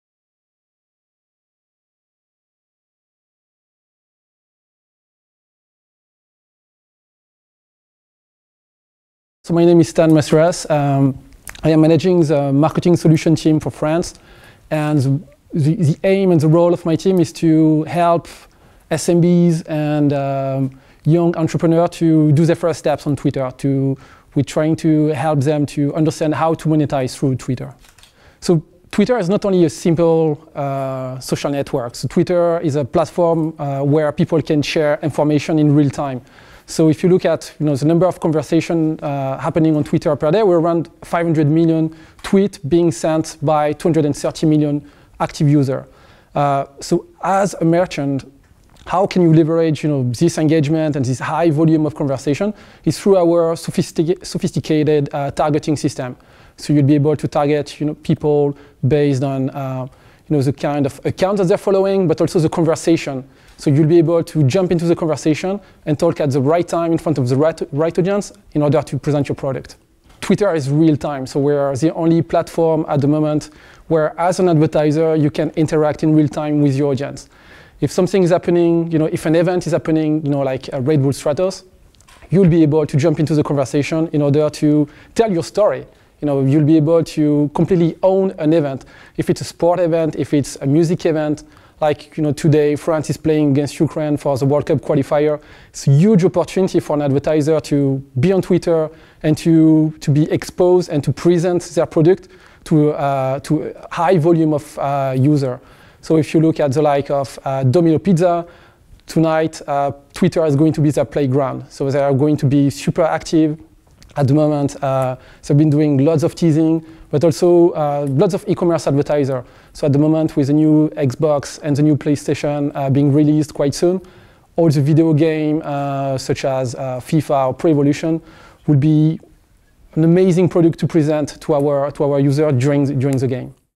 Barcamp 7 - Stan Massueras Interview, Twitter
Stan Massueras interview, Country Manager for Twitter, during the 7th PrestaShop Barcamp in Paris.